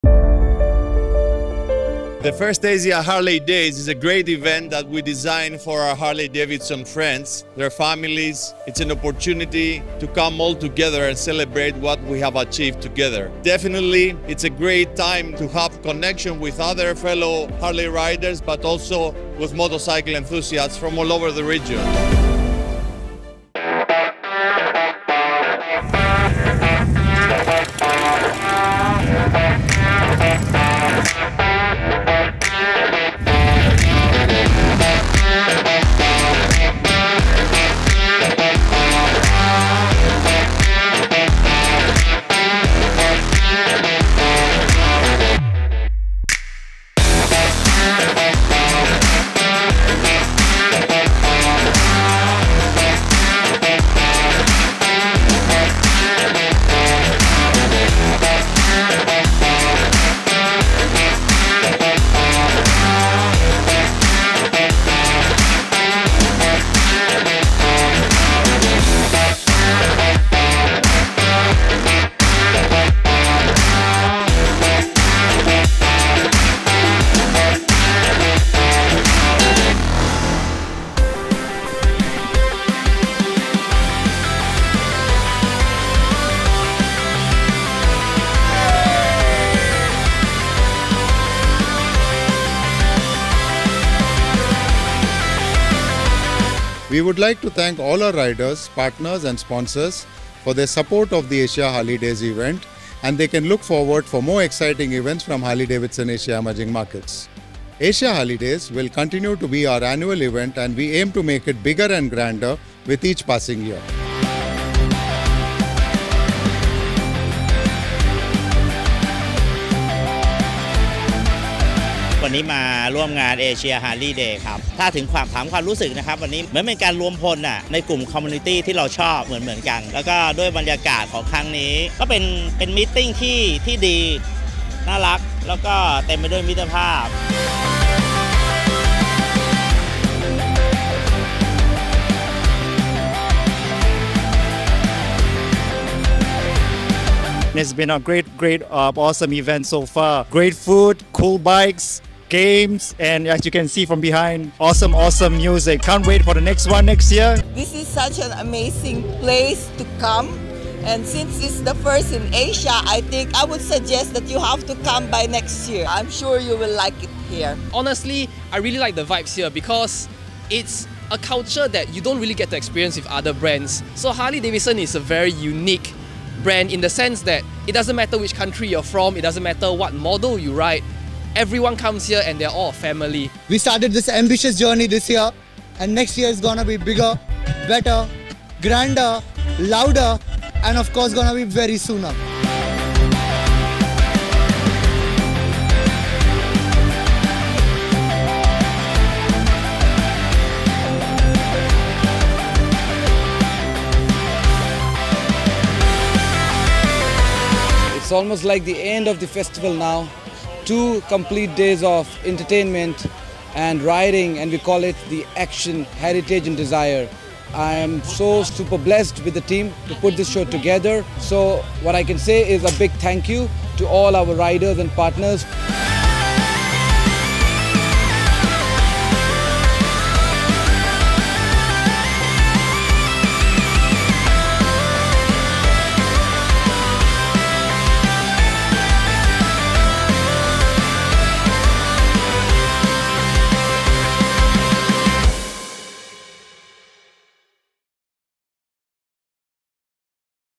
Asia Harley Days 2022 | Harley-Davidson
Let us relive the epic together with the AsiaHarleyDays official wrap-up film.

Thanks to the riders, enthusiasts, H.O.G. chapters, MC clubs, dealer partners and sponsors who came together from over 14 countries & territories.

The 2-day motor-music festival at Cha-am beach, Thailand, with the thrilling performances, music bands, motorcycling activities and award ceremonies enchanted over 1600 participants.

The AHD2022 parade on day 1 was led by management team from Harley-Davidson Motor Company - Dimitris Raptis, Vice-President, Asia Pacific and Sajeev Rajasekharan, Managing Director, Asia Emerging Markets and India.

We have just begun; the next editions will be even bigger and grander.